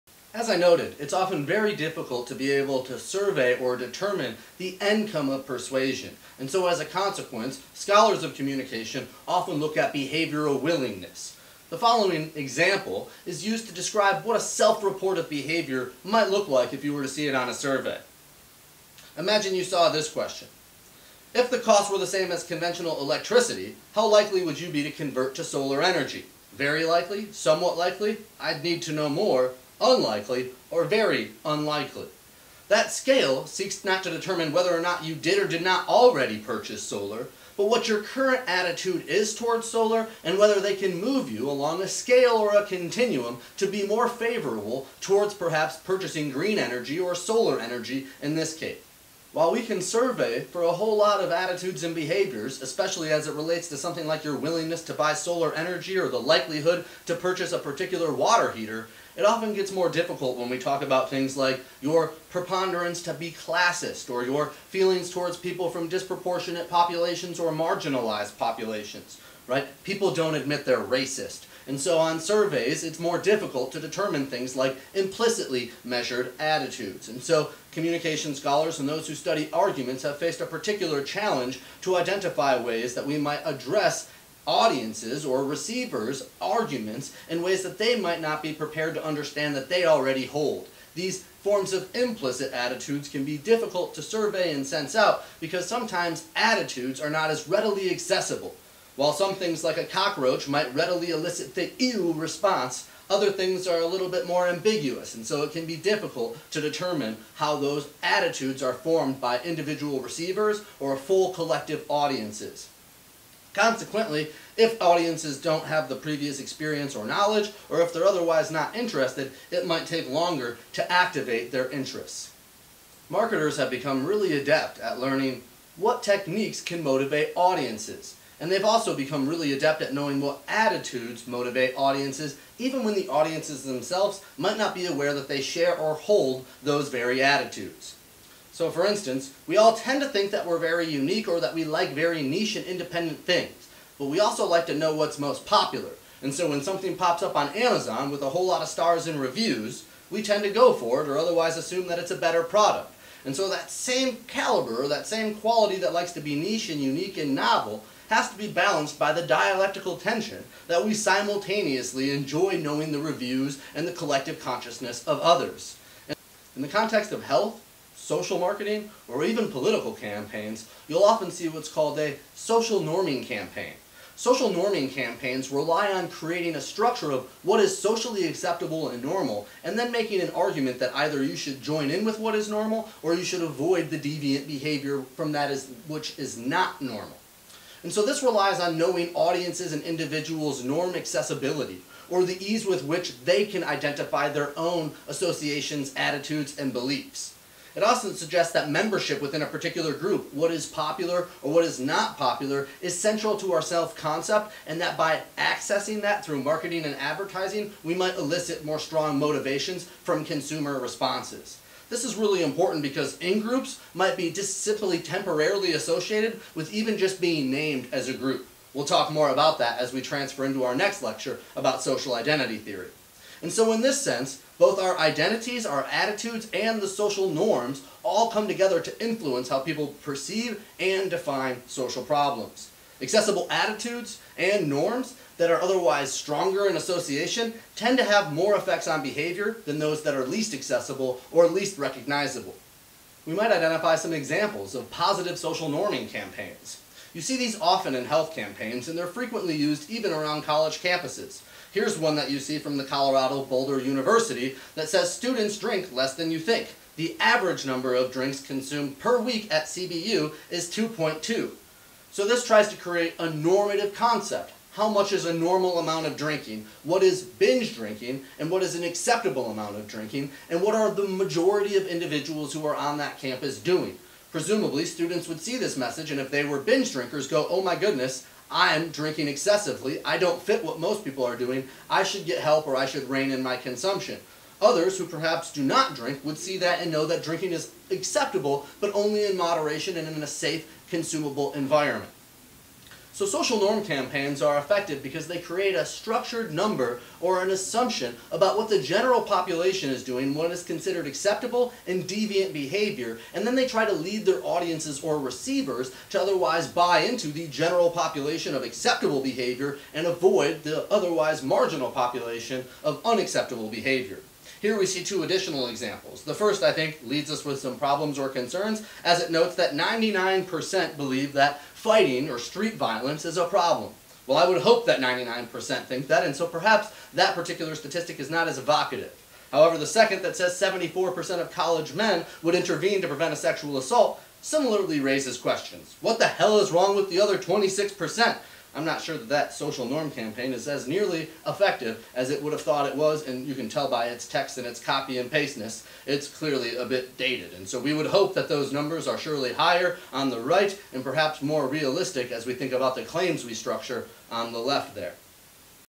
Surveying Attitudes and Attitude (in)Accessibility (2/4)
This video lecture is for UNLV students enrolled in Principals of Persuasion. The video is the second in a series associated with Inoculation Theory. This video describes how scholars of argumentation survey attitude changes and discusses Social Norm Campaigns as a method of persuading audience attitudes.